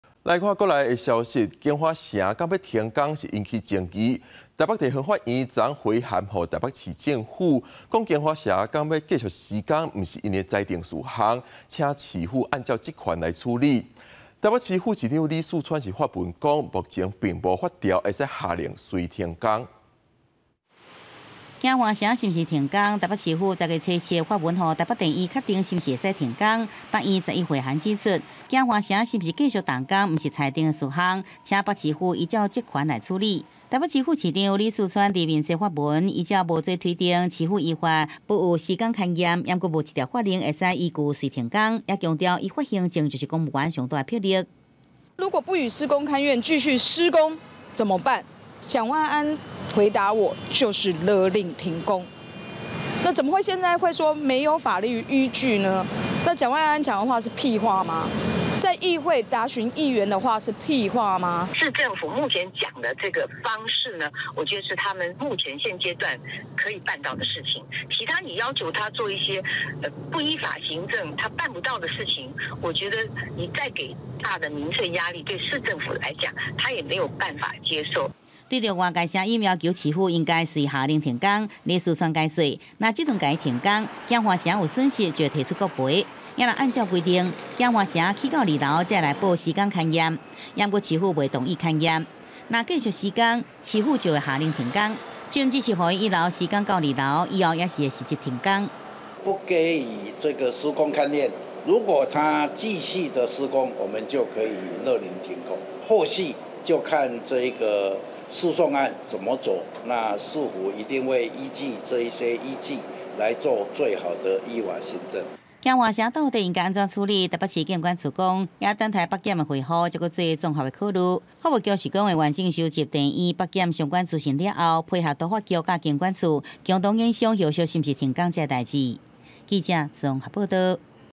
京華城敢欲停工？ 北院：臺北市府著家己決定 | 公視台語台 | 20241012
京華城敢欲停工引起爭議，台北地方法院昨昏回函予台北市政府，講京華城敢欲繼續施工，毋是in的裁定事項，請市府按照職權處理。台北市副市長李四川發文講，目前並無法條會使下令隨停工。

四節新聞 ：　逐工（每天）09:00 / 12:00 / 15:00（拜一至拜五） / 19:00